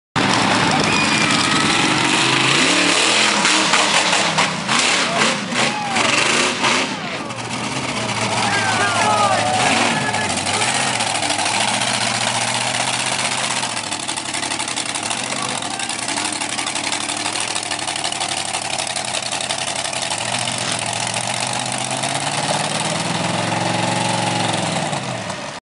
Extreme Monster Truck Tyre Blowout Aberdeen 2012
Extreme Monster Truck Tyre Blowout Extreme Stuntshow Aberdeen 2012 Huge Air Jump Glenn Wise